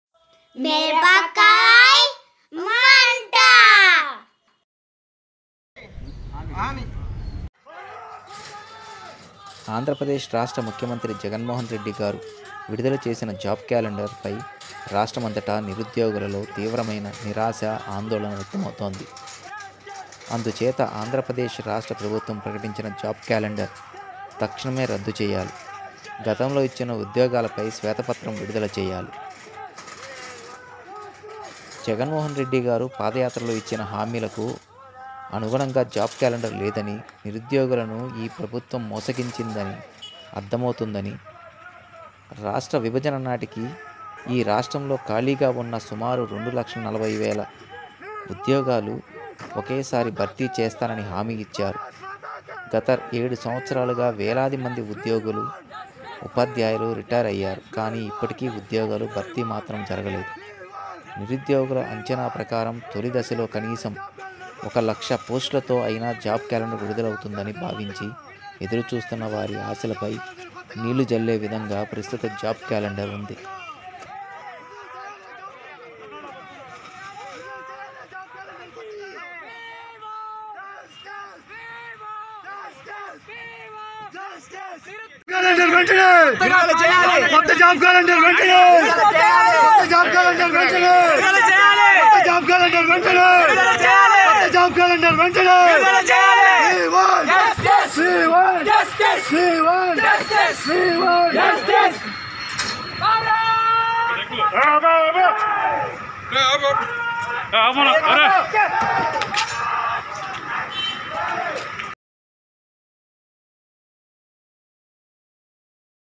మంత్రి బాలినేని ఇంటి ముట్టడి
బీజేవైఎం మంత్రి బాలినేని ఇంటి  ముట్టడించారు